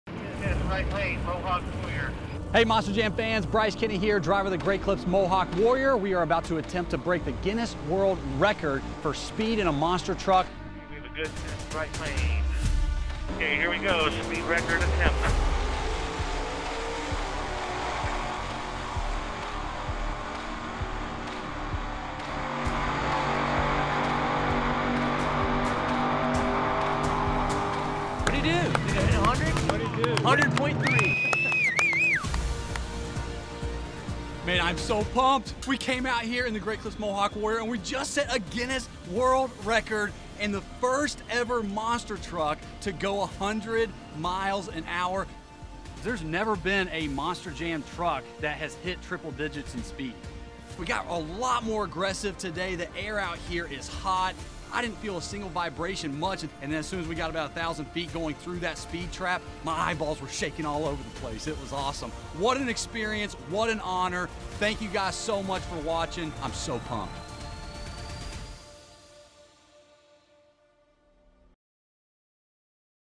Monster Jam Breaking World Records: Fast Speed Attempt
Watch Bryce Kenny driver of the Great Clips Mohawk Warrior official World Record breaking attempt for the fastest speed in a Monster Truck clocking in over 100mph!!